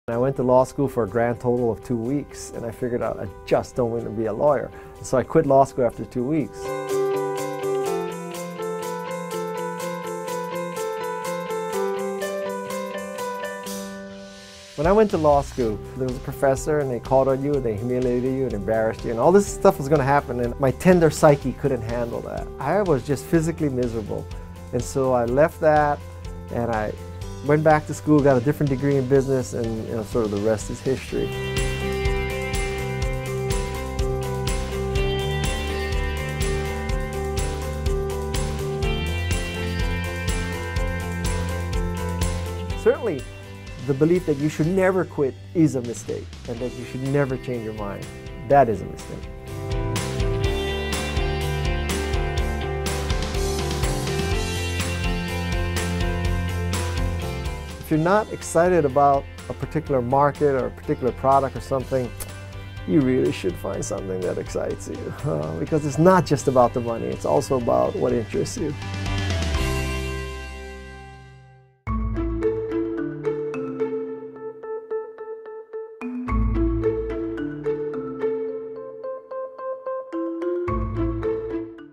Guy Kawasaki Learned This Crucial Career Lesson By Quitting Law School After 2 Weeks | CNBC Make It.
Guy Kawasaki talks to CNBC Make It about why he quit law school after 2 weeks.